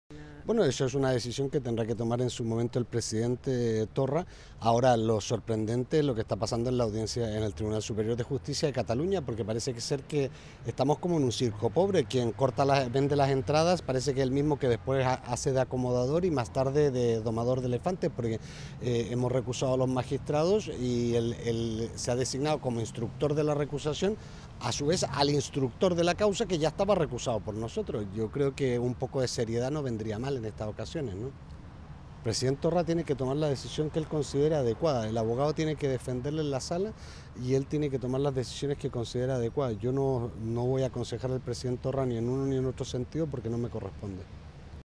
Abogado de Torra critica al TSJC por el juicio de los lazos amarillos
Gonzalo Boye, abogado de Quim Torra, no ha querido desvelar si el president de la Generalitat acudirá al juicio en el que se le juzgará por desobediencia al negarse a retirar los lazos amarillos de la fachada del Palau de la Generalitat ante el Tribunal Superior de Justicia de Cataluña (TSJC) los próximos 25 y 26 de septiembre, aunque ha arremetido contra este tribunal, al que ha pedido "un poco de seriedad" por lo que está ocurriendo en torno al mismo.